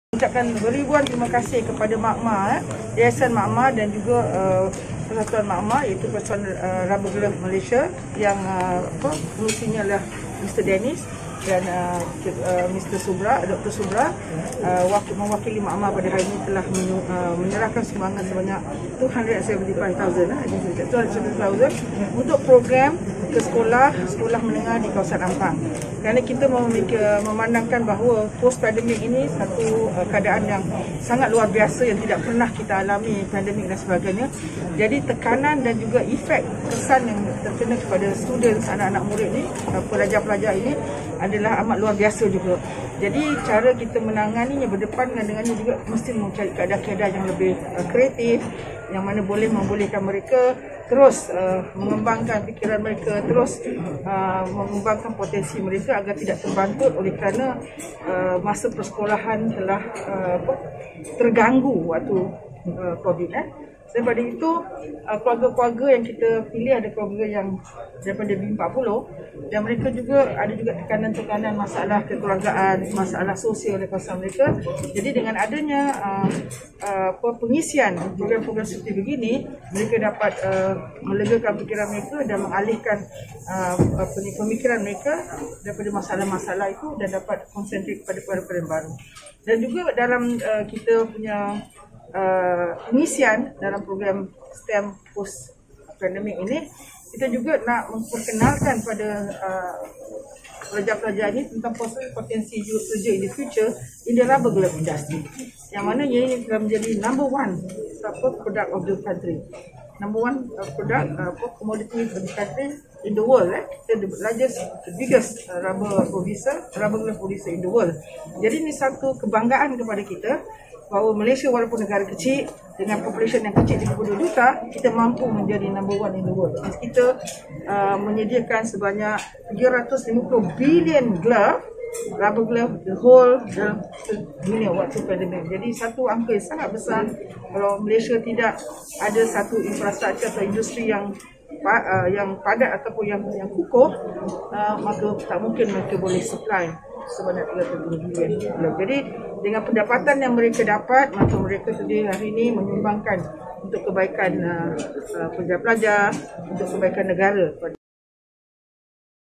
Yayasan Margma Dan KPPK Bantu Program Pendidikan Pasca Pandemik
Menteri Perusahaan Perladangan dan Komoditi YB Datuk Hajah Zuraida
Kamaruddin hari ini menyaksikan penyerahan sumbangan daripada Yayasan
Persatuan Pengeluar Sarung Tangan Getah Malaysia (MARGMA) kepada Pertubuhan Harapan Indah Ampang Selangor di Hotel Grand Barakah, Ampang.

Penyerahan sumbangan ini adalah antara tanggungjawab sosial (CSR) Yayasan MARGMA bagi menyokong pelaksanaan Program Seminar Pendidikan Pasca Pandemik, yang dijalankan khusus untuk Ahli Persatuan Ibu bapa dan Guru (PIBG) bagi 19 buah sekolah terpilih.

Program ini bertujuan untuk mengetengahkan peranan ibu bapa dan guru dalam mendepani kesan pandemik khususnya kepada sistem pendidikan serta kesihatan mental pelajar. Selain itu, seminar ini juga akan menyentuh topik pendidikan STEM (Sains, Teknologi, Kejuruteraan dan Matematik) dan akan dikendalikan pakar-pakar bertauliah dalam bidang pendidikan dan psikologi.

Tema dipilih dalam program ini adalah “Ke Arah Kecemerlangan Sistem Pendidikan dan Kesejahteraan Mental Pelajar”, yang bermatlamat membantu ibu bapa dan guru mendalami pendidikan STEM, seiring dengan keperluan revolusi industri 4.0. Bahkan, program ini juga diyakini dapat membantu ibu bapa dan guru mendalami anjakan corak pengajaran dan pembelajaran terkini.

Program ini dijangka akan dilaksanakan secara berperingkat bermula pada 16
Mei 2022 hingga 31 Oktober 2022.